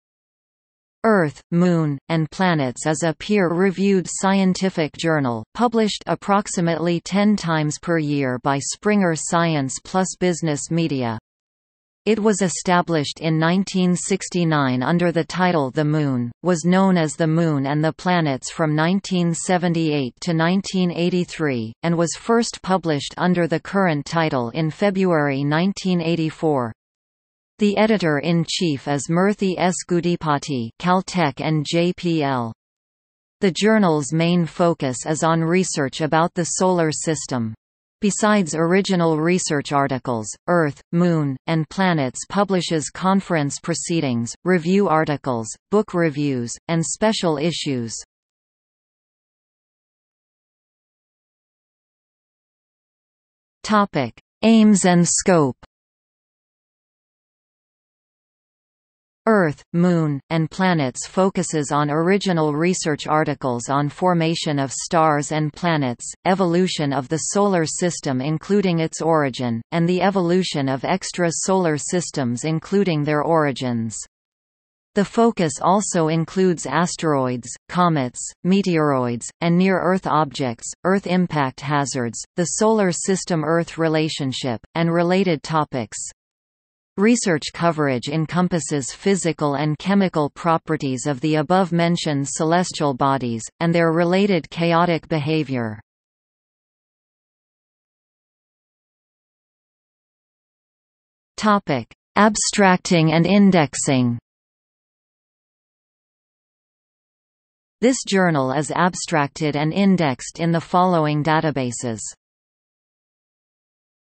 Earth, Moon, and Planets | Wikipedia audio article
00:00:50 Aims and scope
00:01:37 Abstracting and indexing

- Socrates

SUMMARY
Earth, Moon, and Planets is a peer-reviewed scientific journal, published approximately ten times per year by Springer Science+Business Media. It was established in 1969 under the title The Moon, was known as The Moon and the Planets from 1978 to 1983, and was first published under the current title in February 1984. The editor-in-chief is Murthy S. Gudipati (Caltech and JPL). The journal's main focus is on research about the Solar System. Besides original research articles, Earth, Moon, and Planets publishes conference proceedings, review articles, book reviews, and special issues.